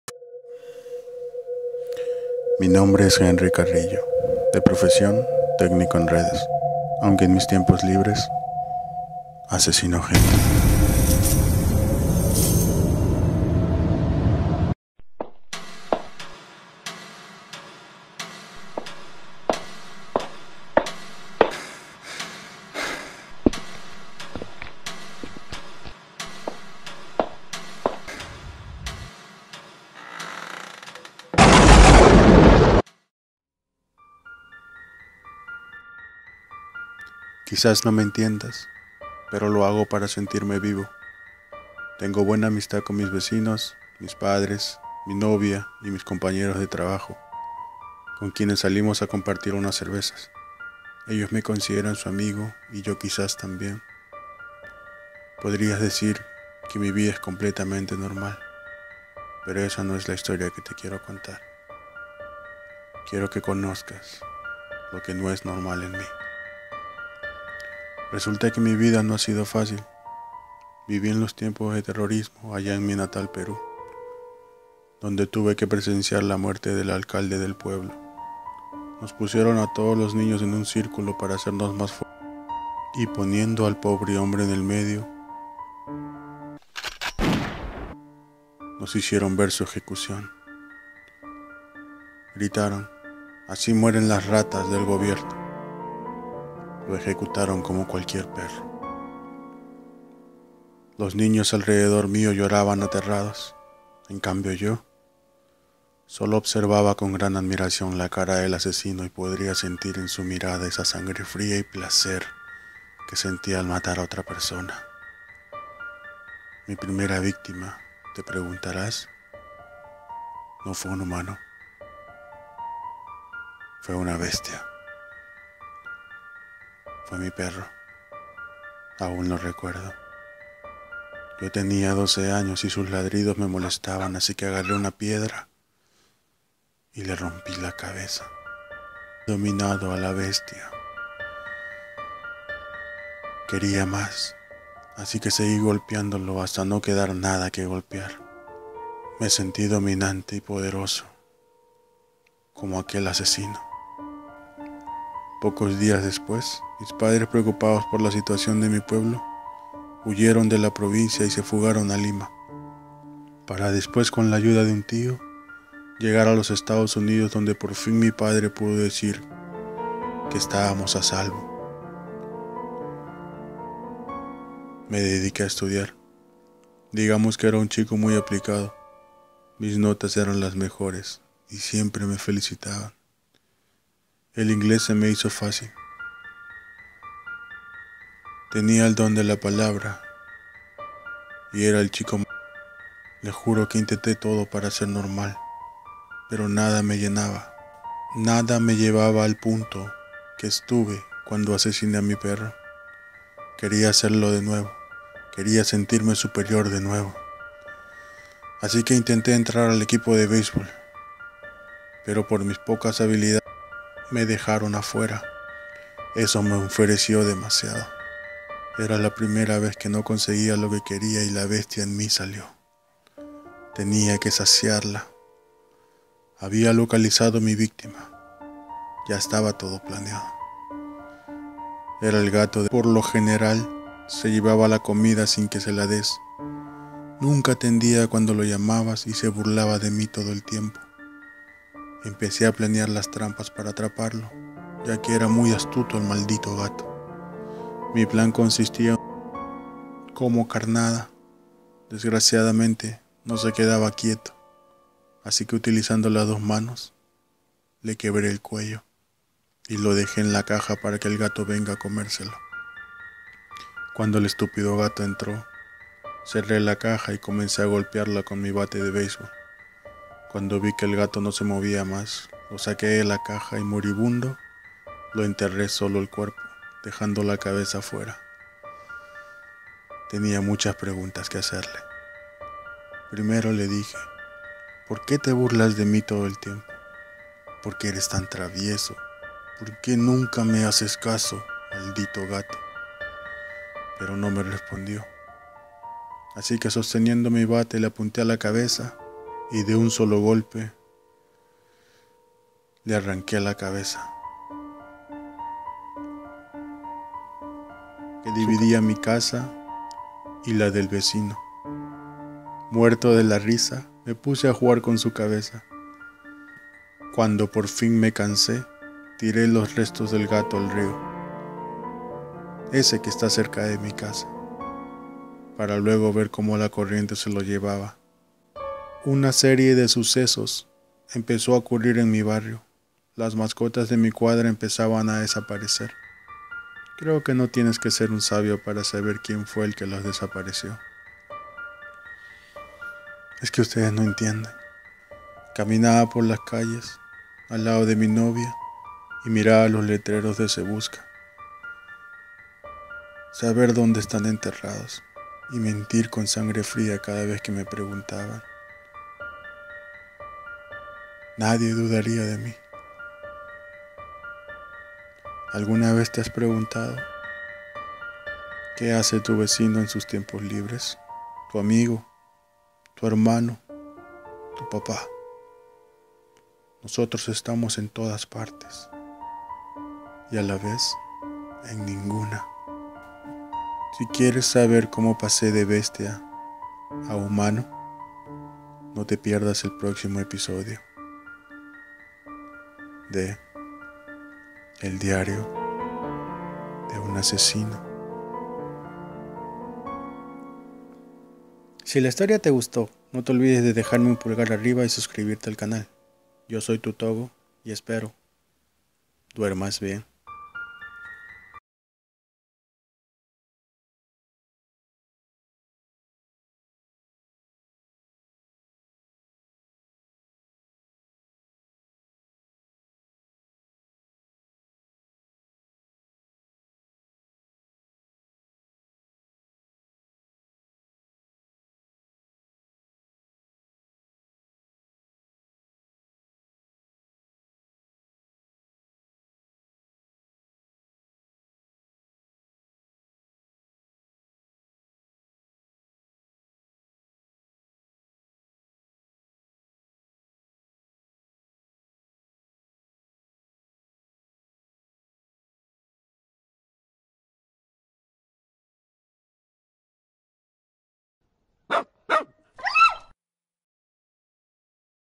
Historias de terror: El Diario De Un Asesino
henry carrillo, un joven que por fuera aparenta ser un muchacho normal y por dentro el infierno corre por sus venas, hara de todo para saciar a la bestia que lleva oculto.. sabes quien esta a tu lado? .. mejor que te lo preguntes una y otra ves...

historia escrita con el fin de entretenerlo..